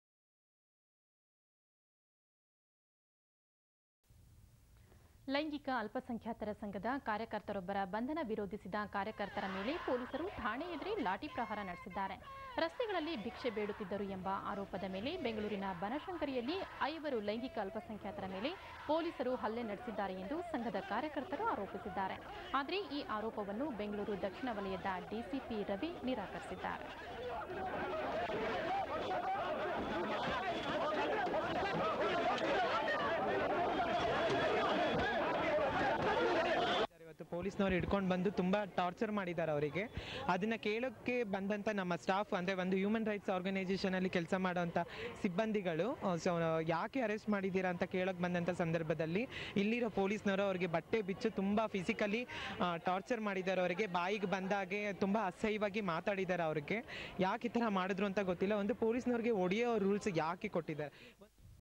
Banashankari Protest
ಬನಶಂಕರಿಯಲ್ಲಿ ಲೈಂಗಿಕ ಕಾರ್ಯಕರ್ತೆಯ ಮೇಲೆ ಹಲ್ಲೆ ಮತ್ತುಬಂಧನ ಖಂಡಿಸಿ ಪ್ರತಿಭಟನೆ